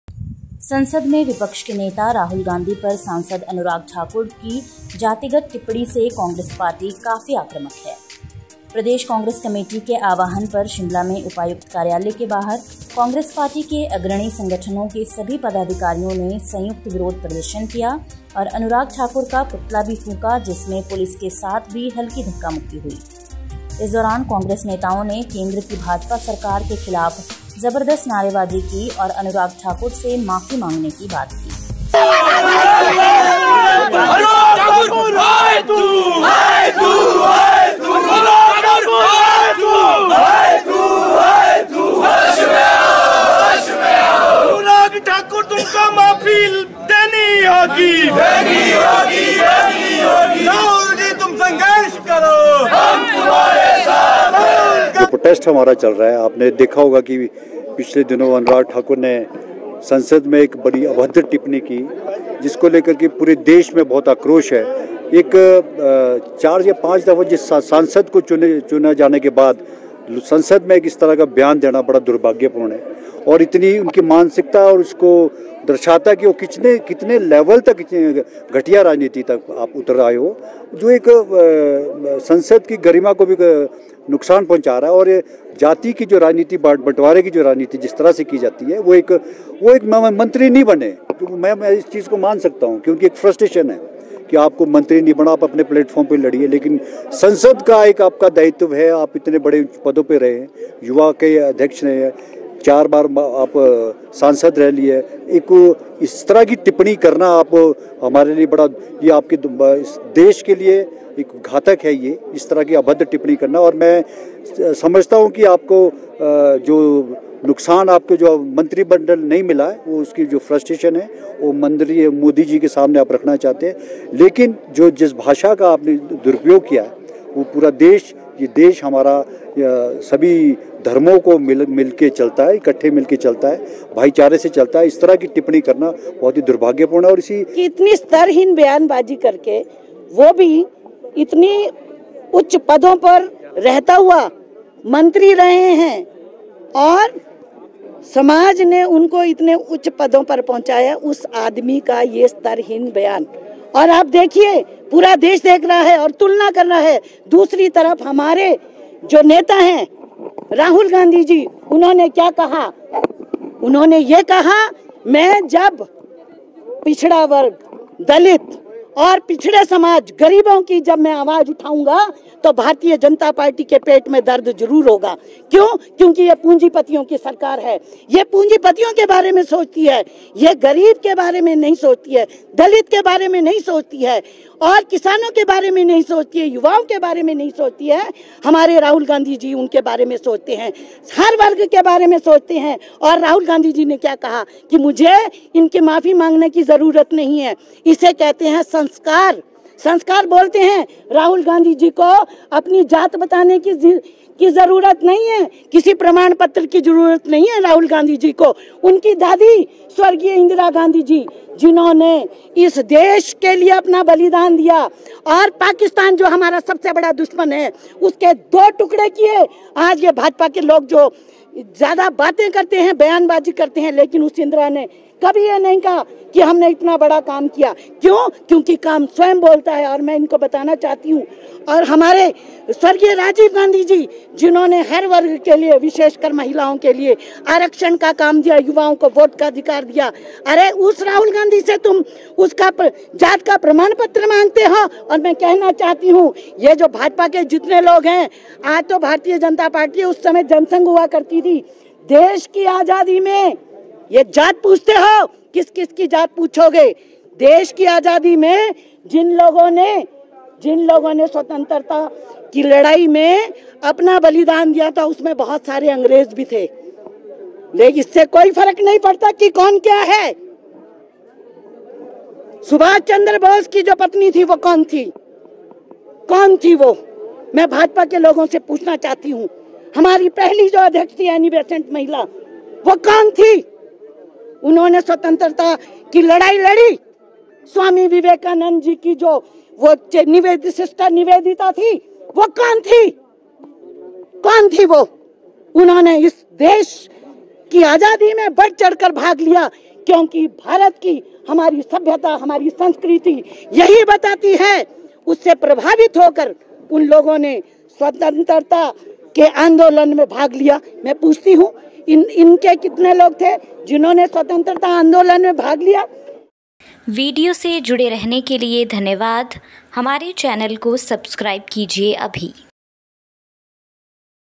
राहुल गांधी पर अनुराग ठाकुर की जातिगत टिप्पणी पर भड़की कांग्रेस, शिमला में फूंका पुतला
संसद में विपक्ष के नेता राहुल गांधी पर सांसद अनुराग ठाकुर की जातिगत टिपण्णी से कांग्रेस पार्टी काफी आक्रमक है। प्रदेश कांग्रेस कमेटी के आह्वान पर शिमला में उपायुक्त कार्यालय के बाहर कांग्रेस पार्टी के अग्रणी संगठनों के सभी पदाधिकारियों ने संयुक्त विरोध प्रदर्शन किया और अनुराग ठाकुर का पुतला भी फूंका जिसमें पुलिस के साथ भी हल्की धक्कामुक्की हुई।।इस दौरान कांग्रेस नेताओं ने केंद्र की भाजपा सरकार के खिलाफ जबरदस्त नारेबाजी की और अनुराग ठाकुर से माफी मांगने की मांग की।

The Congress party is quite aggressive due to the casteist remark of MP Anurag Thakur on the Leader of Opposition Rahul Gandhi in the Parliament. On the call of the State Congress Committee, all the office bearers of the leading organizations of the Congress Party held a joint protest outside the Deputy Commissioner's office in Shimla and also burnt the effigy of Anurag Thakur, in which there was a mild scuffle with the police. During this, the Congress leaders raised strong slogans against the BJP government at the Center and demanded an apology from Anurag Thakur.